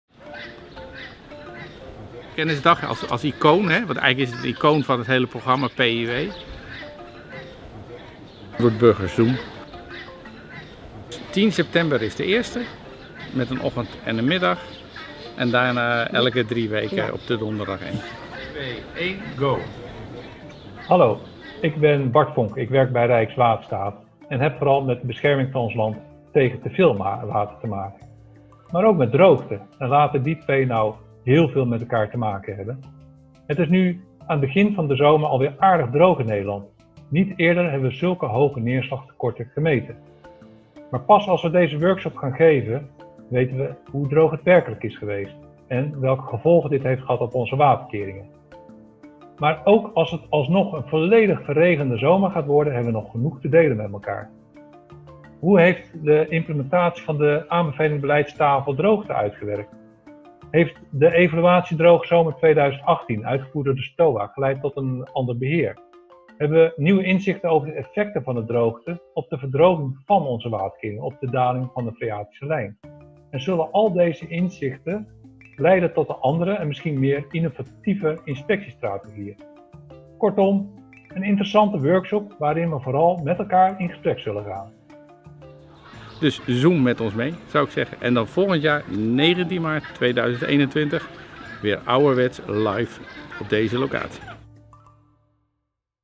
Waterveiligheid | Teaser Kennisdag Inspectie Waterkeringen, thema droogte | 2020
Niet eerder hadden we in 2020 zo vroeg in het jaar zulke hoge neerslagtekorten. Maar pas als we deze online workshop gaan geven, eind oktober, zullen we weten hoe de droogte zich dit jaar heeft ontwikkeld en welke gevolgen dat heeft gehad. Ook als het alsnog een volledig verregende zomer gaat worden, hebben we genoeg te delen met elkaar.

Hoe heeft de implementatie van de aanbevelingen beleidstafel Droogte ons geholpen? Heeft de 'Evaluatie droge zomer 2018 waterkeringen' (STOWA 2019-10) geleid tot ander beheer? Hebben we nieuwe inzichten over de effecten van droogte op de verdroging van (daling freatische lijn) in onze waterkeringen. Zullen deze inzichten leiden tot andere en misschien meer innovatieve inspectiestrategieën? Kortom: neem deel aan deze workshop over droogte in het kader van de Kennisdag inspectie Waterkeringen 2020, op 29 oktober 2020, waarin we vooral het gesprek met elkaar aan zullen gaan.